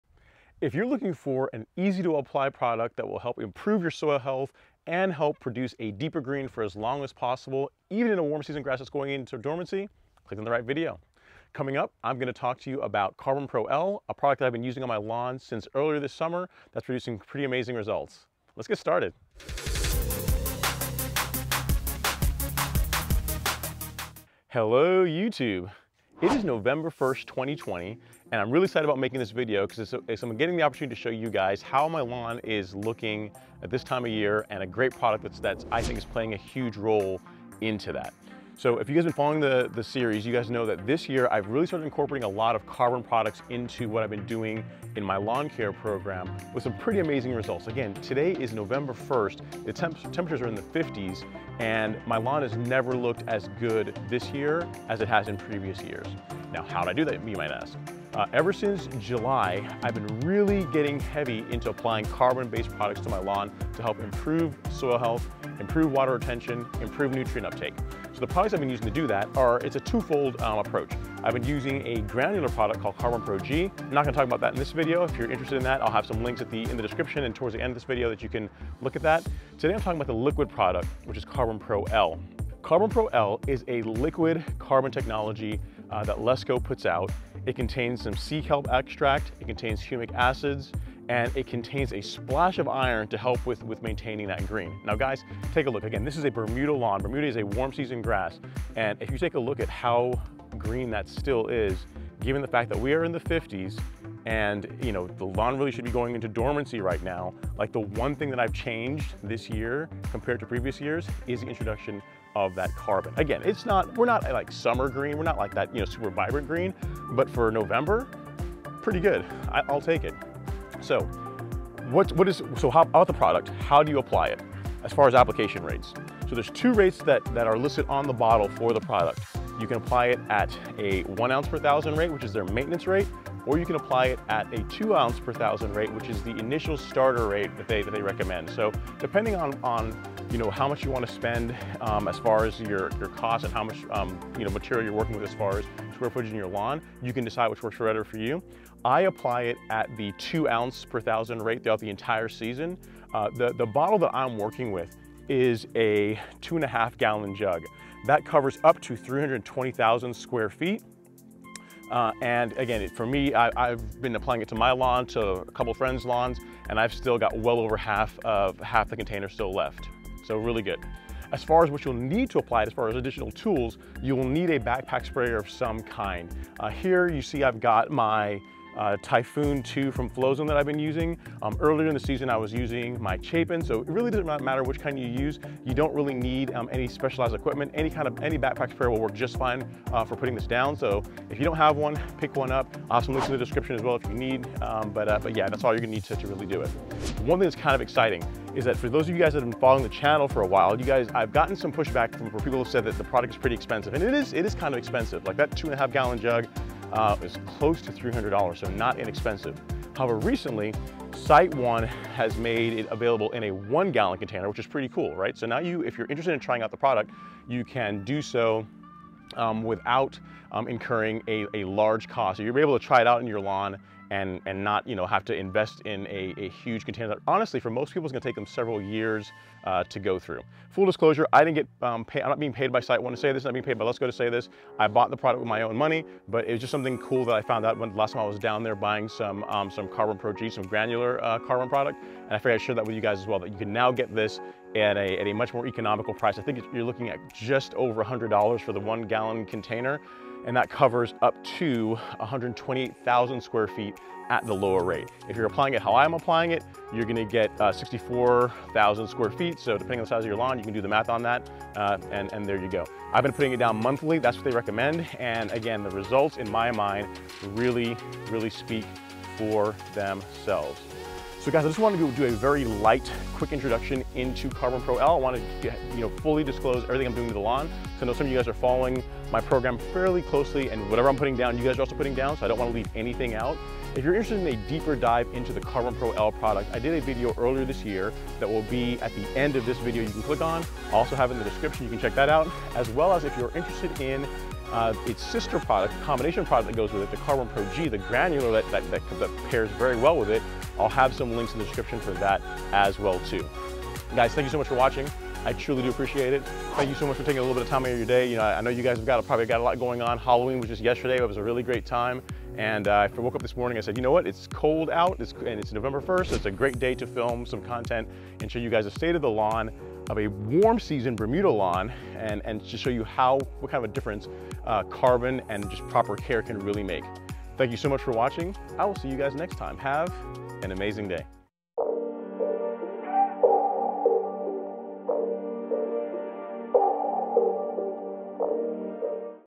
CarbonPro-L - Liquid Carbon for a Greener Lawn
Applying carbon to your lawn in the fall can help produce deep green grass as your lawn transitions. I've been using it in my lawn for the past 5 months and I'm getting some pretty amazing results from it. This video was filmed on November 1st 2020 and the lawn still looks very green and lush. In previous years, my Bermuda grass lawn would already be in dormancy. The fall lawn carbon products I've been making use of are CarbonPro-G and CarbonPro-L. In this video I discuss the liquid carbon product; CarbonPro-L

Lesco CarbonPro-L is an organic fertilizer / soil amendment that conditions the soil and provides many benefits for Bermuda grass and Zoysia grass lawn. It's a liquid soil amendment that contains humic acid, kelp extract and other microbial that conditions the soil to improve plant health. CarbonPro also contains iron as a micronutrient to help improve turf green up. It's a liquid organic fertilizer.

Video Contents ---
0:00 Introduction
0:59 Carbon Added to Lawn Care Program
1:20 CarbonPro-L Contents
1:37 Product Results
2:04 Application Rates
2:38 Lawn Coverage and Tools Needed
3:20 Product Price and Size Options
3:50 Where to Get
4:43 Detailed CarbonPro-L Review

Vlogging Lens